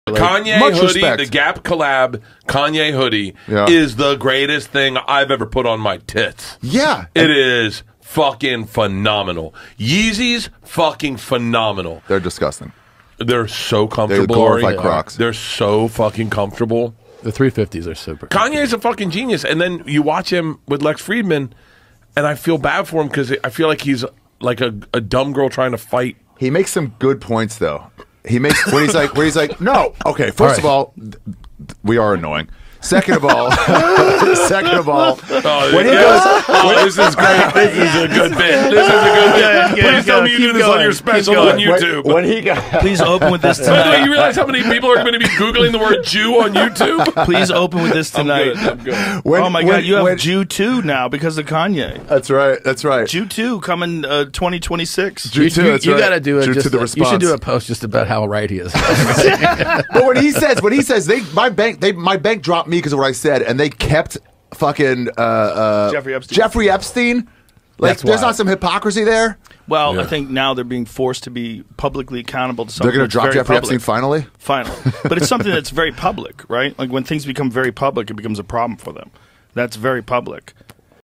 Joe Rogan & Friends - The Hypocrisy of Banks dropping Kanye!
Joe Rogan and friends discuss Kanye West or Ye, and the banks refusing his money.

Credit Joe Rogan